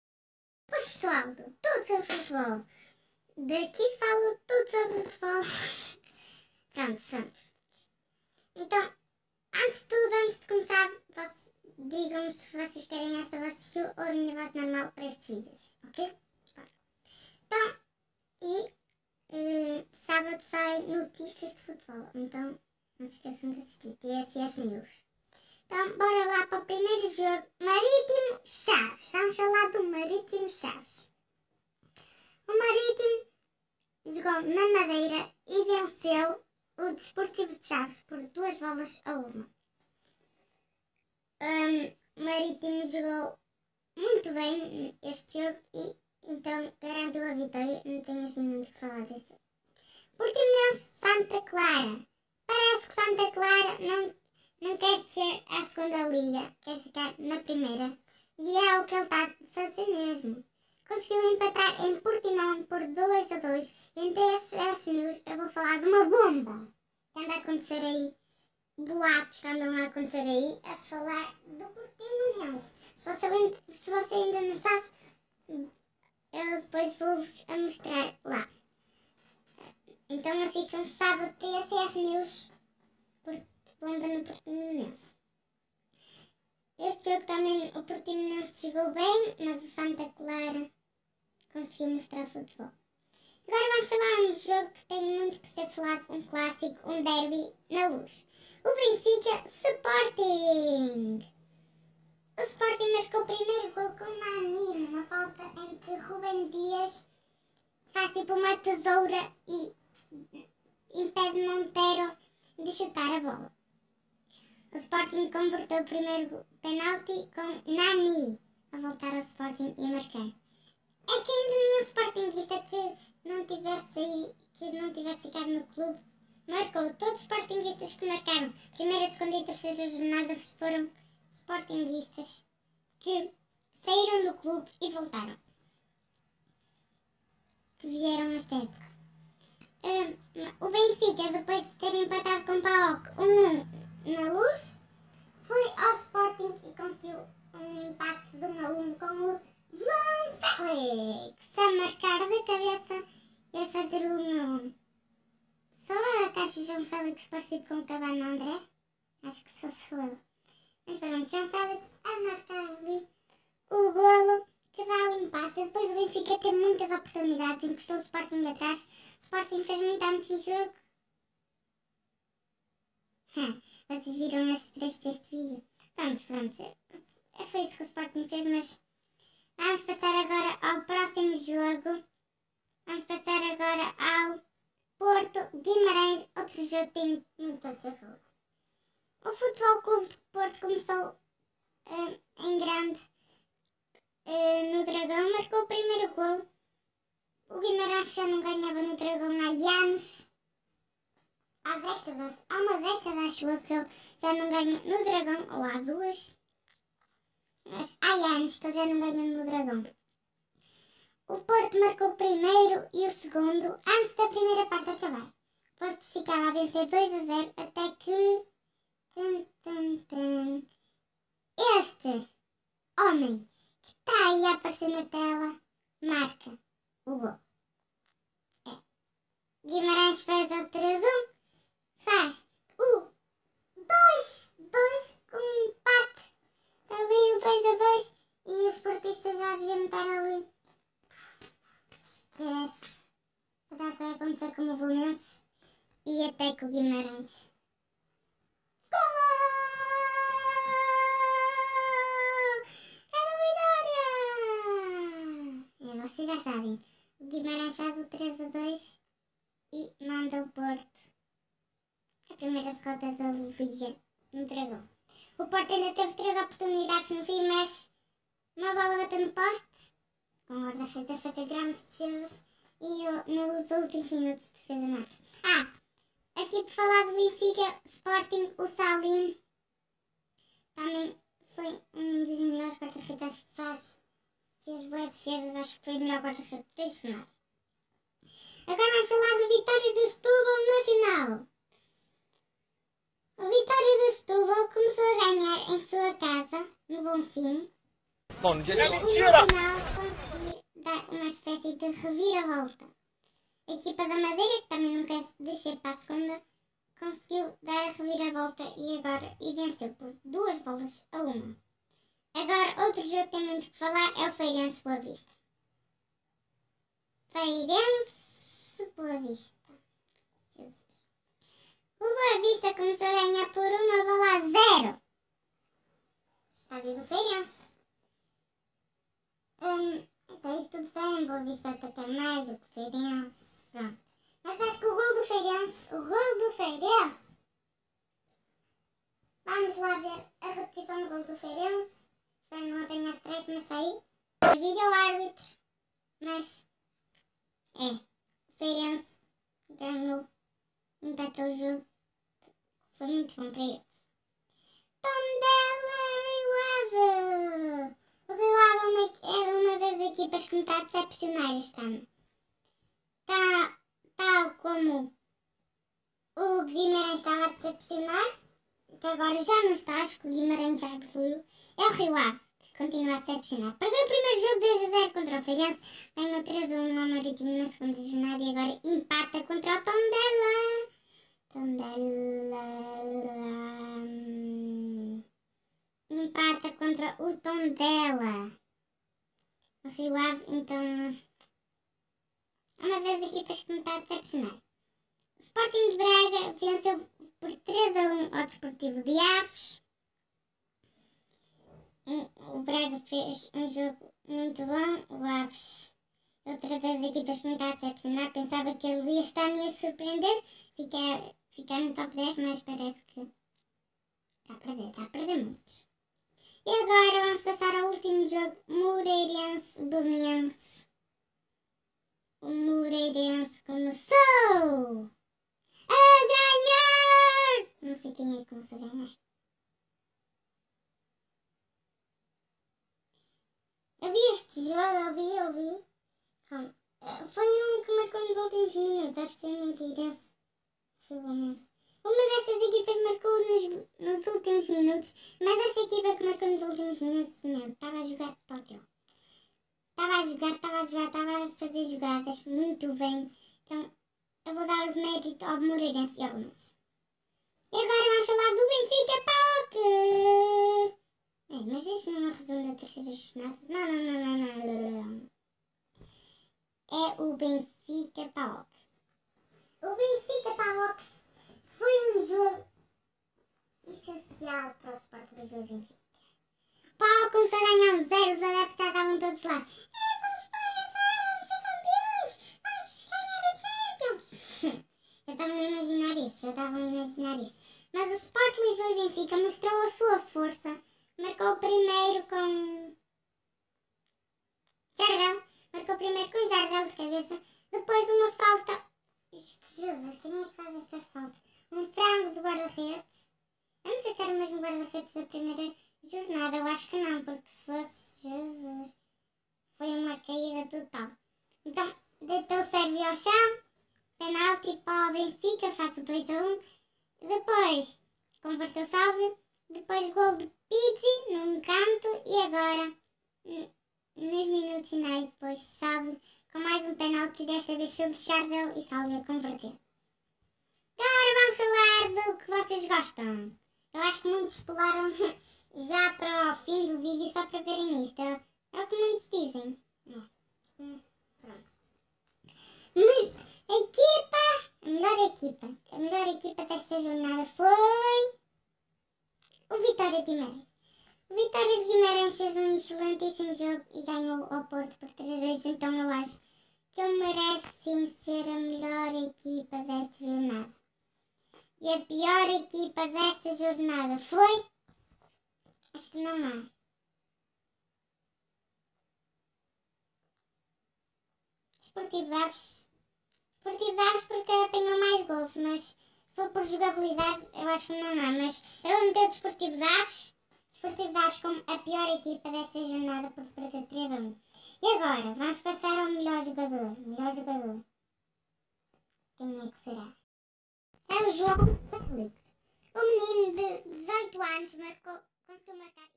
Resumo da 3° jornada - Dérbi de Lisboa , reviravolta no Dragão
Guimarães dá reviravolta no Dragão  frente ao Porto
Benfica empata na Luz com o Sporting